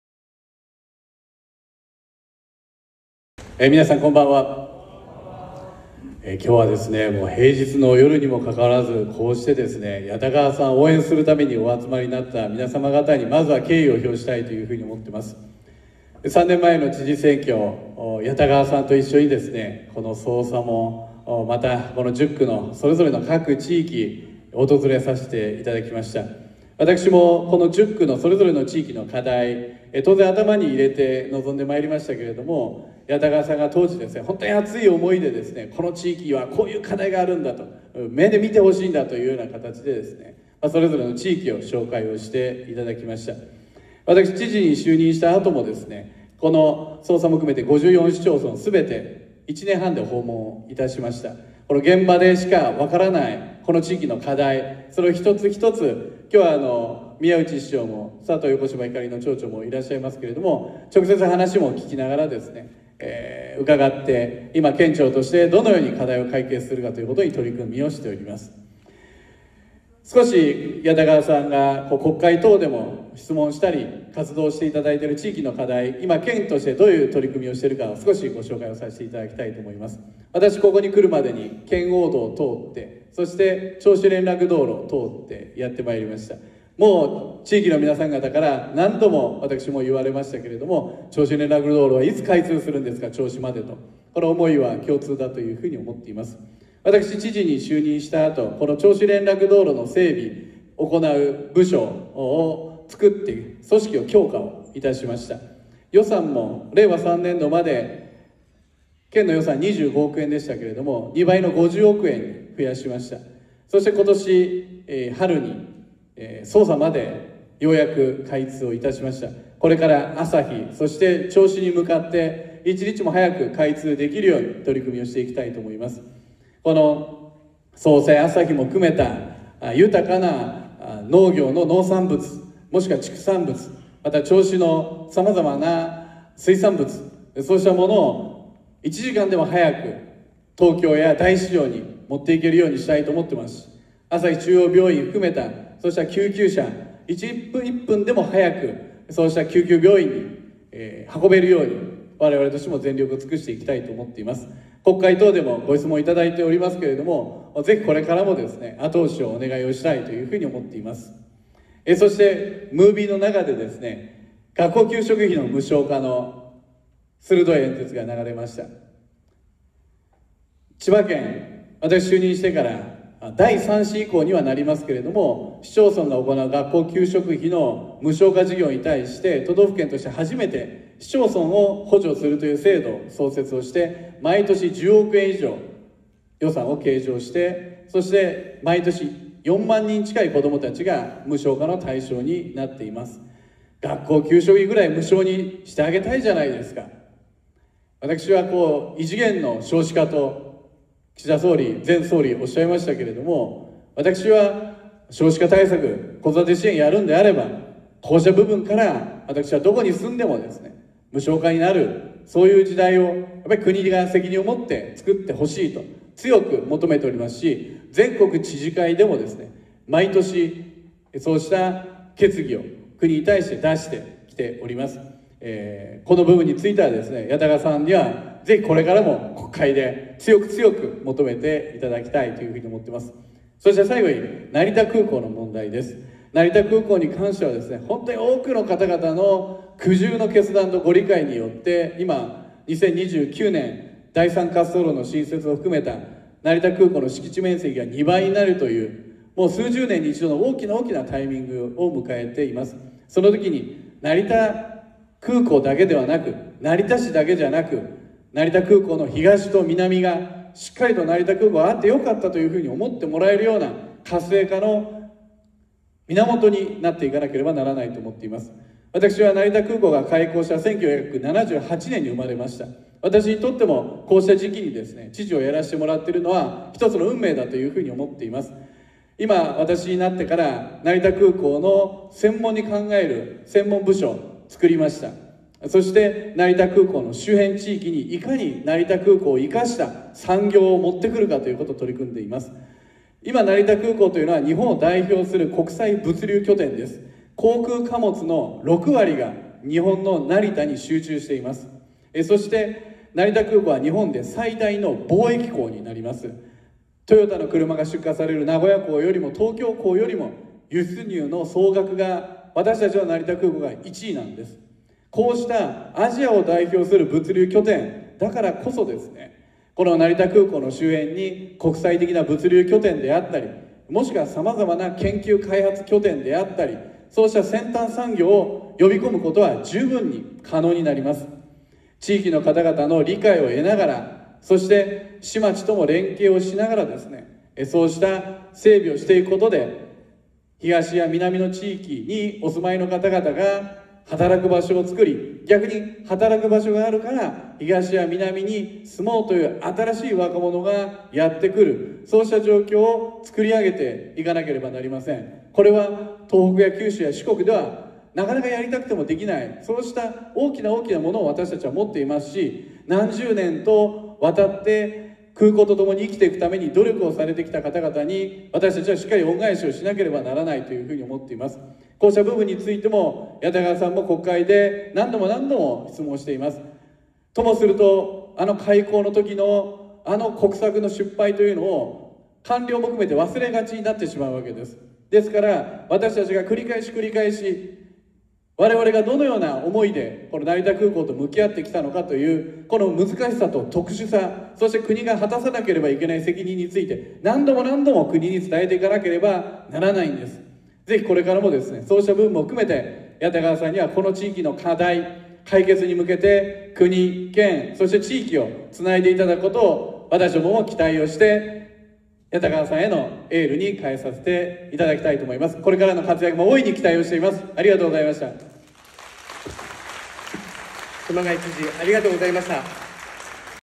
熊谷俊人千葉県知事の応援演説 10/24 谷田川はじめ 個人演説会＠匝瑳より
熊谷知事のお話にある「ムービー」動画はこちらです